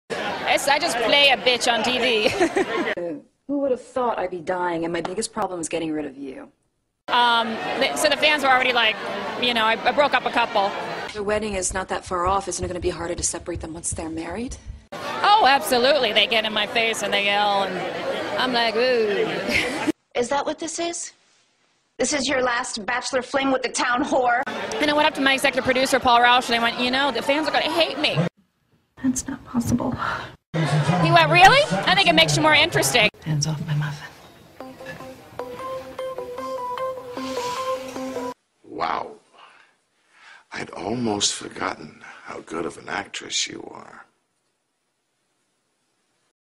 CC / Olivia Spencer quickie
I was watching the Crystal Chappell Interview at  the 2010 Daytime Emmy Awards and I thought it was SO funny, so I had this idea :) I called it a quickie because it's so short lol

With Portuguese and English captions :)